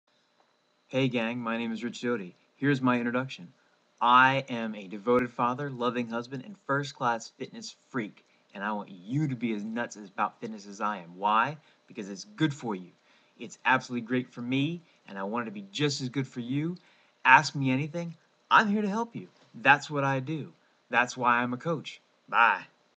Richard Doty's introduction
A brief intro of me.  A 42 year old fitness nut!